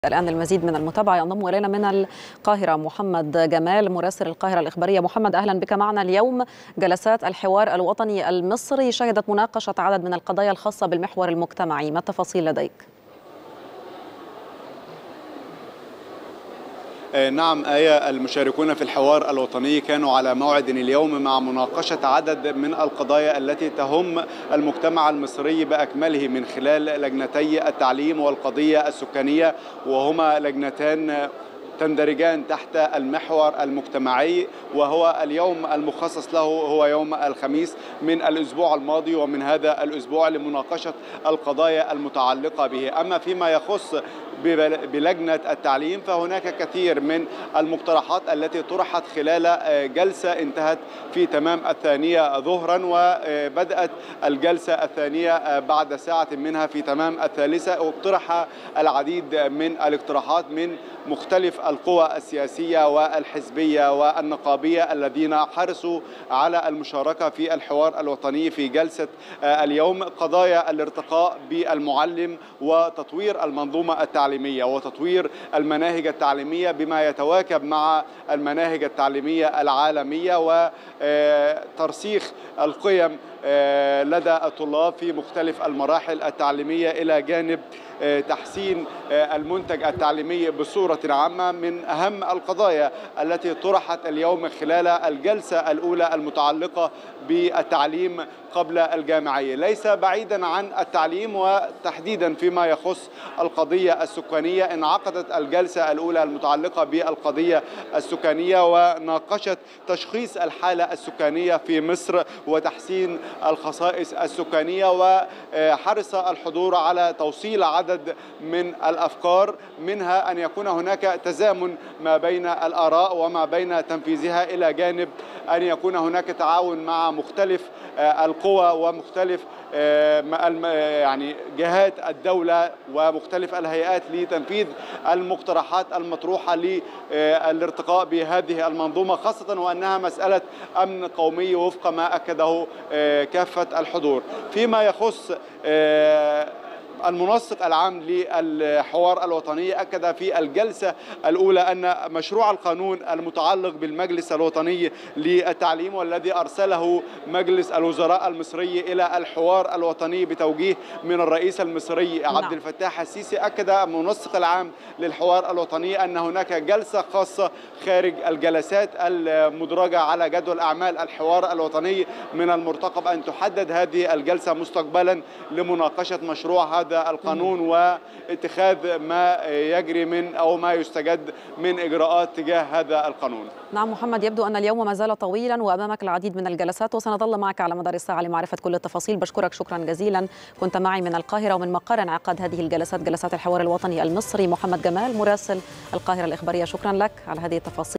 مراسلنا يكشف تفاصيل جلسات المحور المجتمعي بالحوار الوطني المصري بشأن ملف التعليم والسكان
مراسلنا محمد جمال يكشف تفاصيل جلسات المحور المجتمعي بالحوار الوطني المصري بشأن ملف التعليم والسكان

25/5/2023